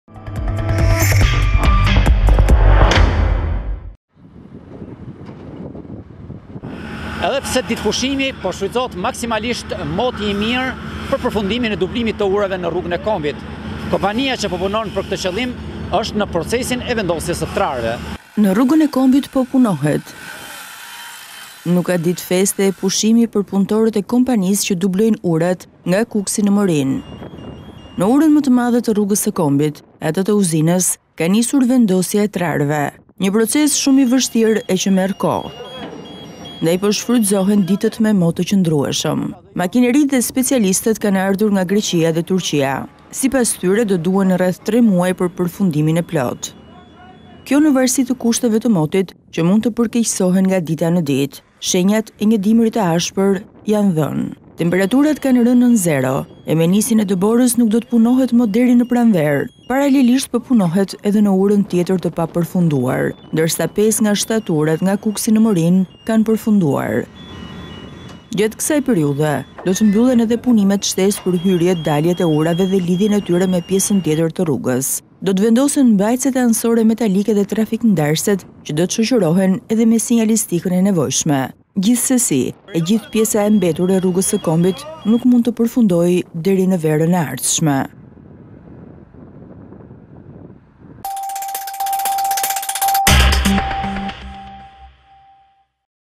Ne Rrugen e Kombit vazhdojne punimet per dublimin e urave | ABC News Albania
Në Rrugën e Kombit vazhdojnë punimet për dublimin e urave dhe lidhjen e tyre. Në terren janë punëtorët e firmës e cila po angazhohet me ritme të shpejta dhe brenda një kohe të shkurtër, investimi do të përfundojë.

Në Rrugën e Kombit po punohet. Nuk ka ditë feste e pushimi për punëtorët e kompanisë që dublojnë urat nga Kukësi në Morinë. Në urën më të madhe të Rrugës së Kombit atë të Uzines, ka nisur vendosja e trarëve. Një proces shumë i vështirë e që merr kohë. Ndaj po shfrytëzohen ditët me mot të qëndrueshëm. Makineritë dhe specialistët kanë ardhur nga Greqia e Turqia.

Sipas tyre do duhen rreth tre muaj për përfundimin e plotë. Kjo në varësi të kushteve të morit që mund të përkeqësohen nga dita në ditë. Shenjat e një dimri të ashpër janë dhënë. Temperaturat kanë rënë nën zero e me nisjen e borës nuk do punohet më deri në pranverë. Paralelisht po punohet edhe në urën tjetër të papërfunduar, ndërsa pesë nga shtatë urat nga Kukësi në Morinë kanë përfunduar.

Gjatë kësaj periudhe do të mbyllen edhe punimet shtesë për hyrje-daljet e urave dhe lidhjen e tyre me pjesën tjetër të rrugës. Do të vendosen mbajtëset anësore metalike e trafikndarëset që do të shoqërohen edhe me sinjalistikën e nevojshme. Gjithësesi e gjithë pjesa e mbetur e Rrugës se Kombit nuk mund të përfundojë deri në verën e ardhshme./abcnews.al

Përgatiti: Bujar Muçmata gazetar i ABC News, Kukës